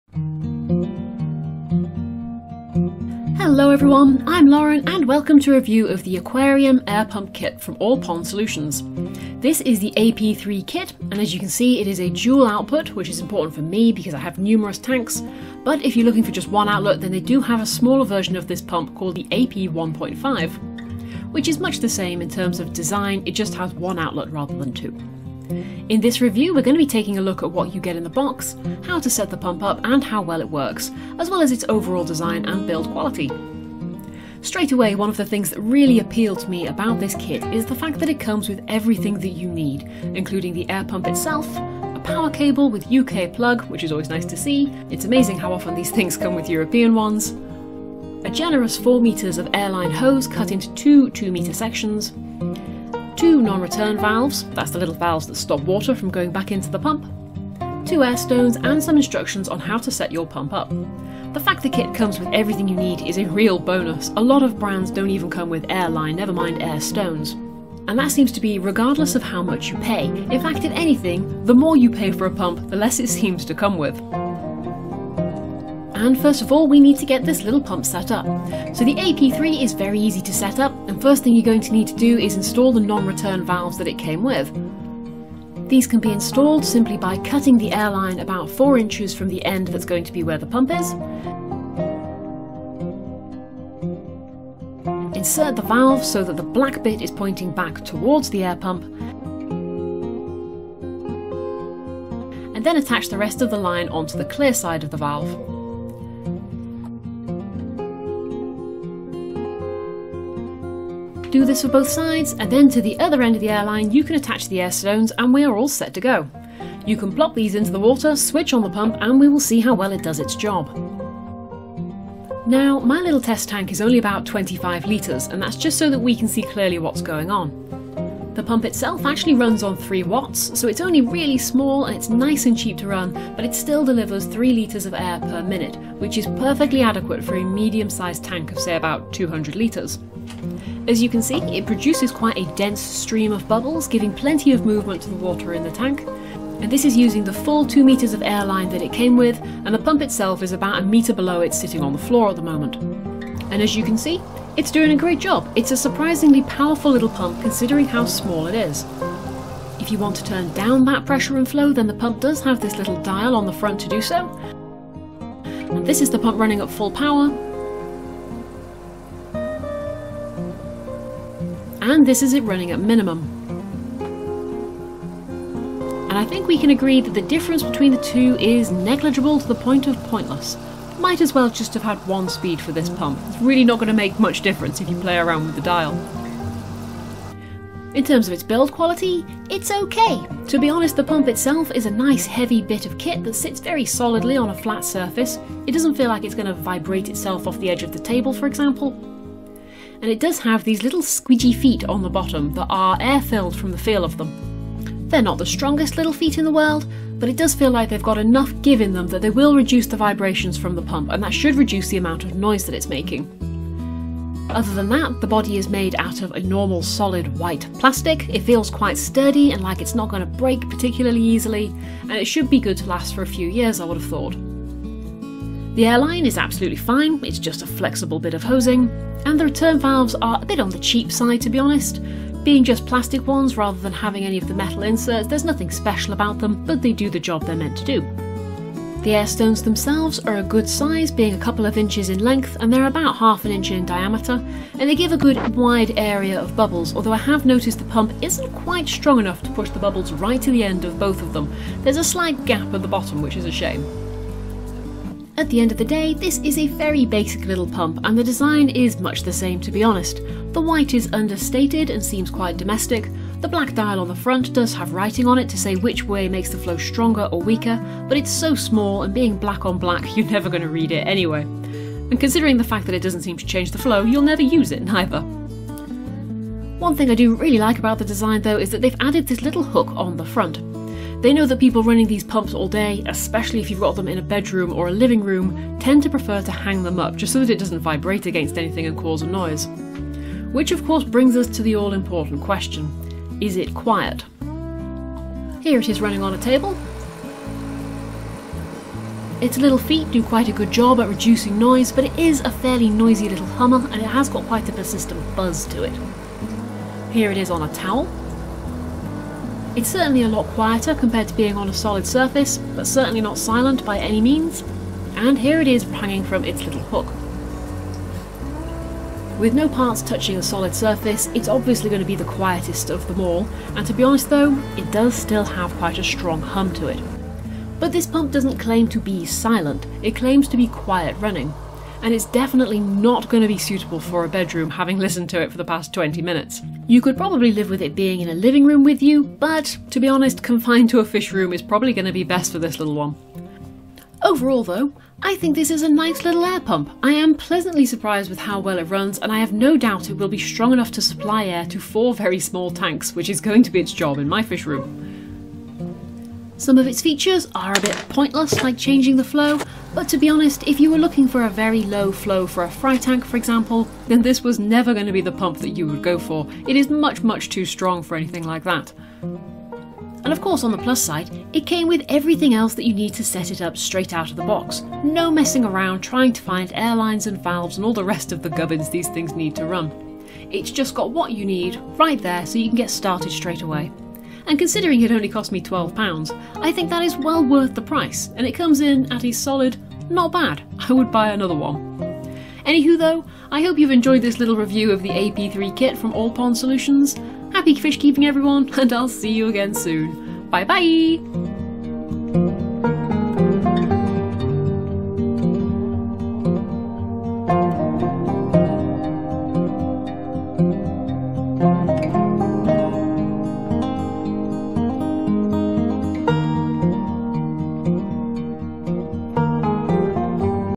Ap-3 All Pond Solutions Air Pump Kit Review - Perfect Air Pump for Medium Aquariums
The A.P-3 kit is an air pump from All Pond Solutions and is ideal for medium to larger tanks up to about 200 litres. It's a nice solid little air pump that feels like it will stand the test of time and produces a good stream of bubbles for aerating/decorating your tank. Be warned though - it's certainly not the quietest of air pumps so you wouldn't want to set this up in a bedroom!

Ap-3 Kit